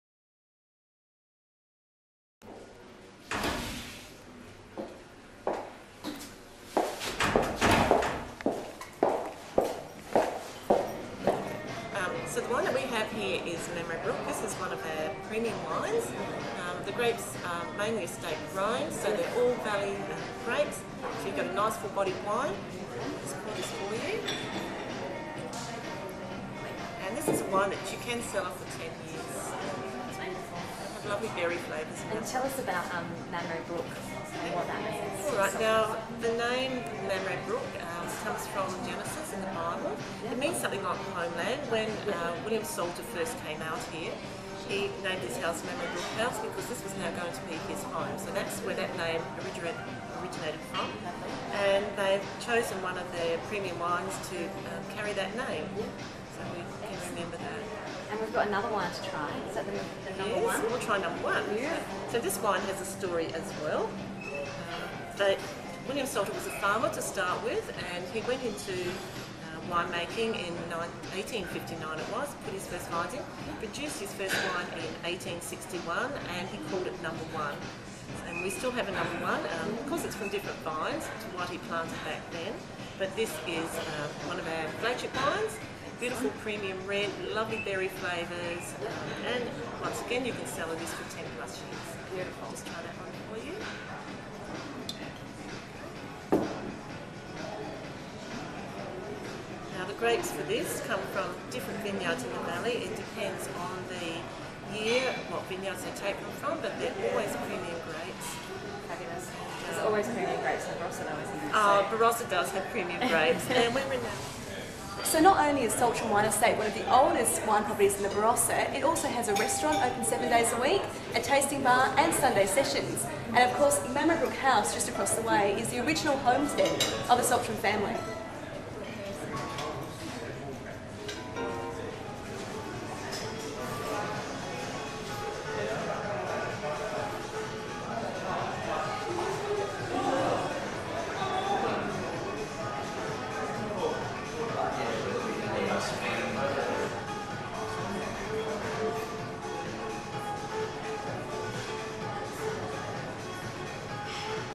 Saltram Wine Estate: The Barossa
Jaci and Paige head out on their Roadtrip in the Barossa to give you a sneak peak of the fabulous offering at Saltram Wine Estate in Angaston.

It's all about the senses at Saltram's... You can enjoy a coffee in the morning sun on their balcony, celebrate a party or gathering in one of their function areas, enjoying a sumptuous dinner with family and friends, or simply stop off for a tasting and a light lunch.